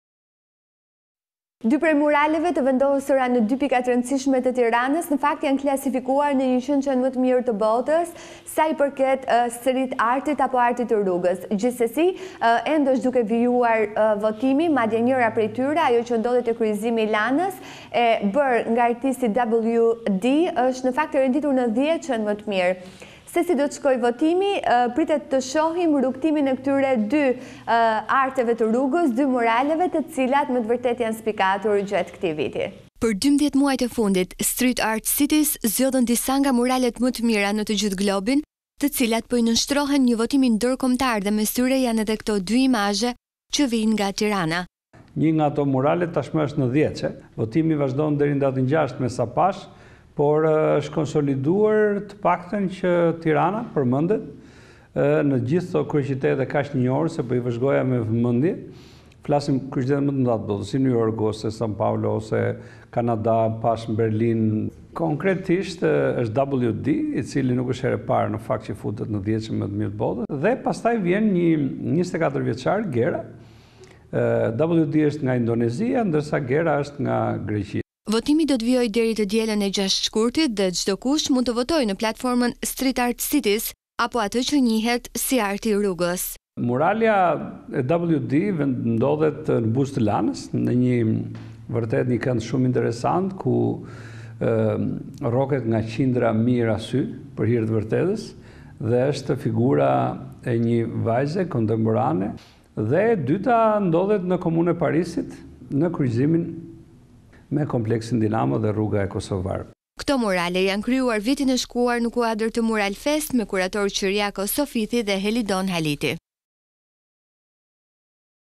Top Channel/ Muralet e Tiranës mes më të mirave! Dy prej tyre mes 100 muraleve më të bukura në botë
Dy prej muralave të vendosura në dy pika të rëndësishme të tiranës janë klasifikuar në 100 më të mirë të botës sa i përket artit të rrugës. Ende është duke vijuar votimi , madje njëra prej tyre është renditur në dhjetëshen më të mirë. Se di do të shkojë rrugëtimi pritet të shohim rrugëtimin e tyre që me të vërtetë janë spikatur gjatë këtij viti.

Për 12 muajt e fundit, Street Art Cities zgjodhën disa nga muralet më të mira në të gjithë globin, të cilat po I nënështrohen një votimi ndërkombëtar dhe mes tyre janë këto dy imazhe që vijnë nga Tirana.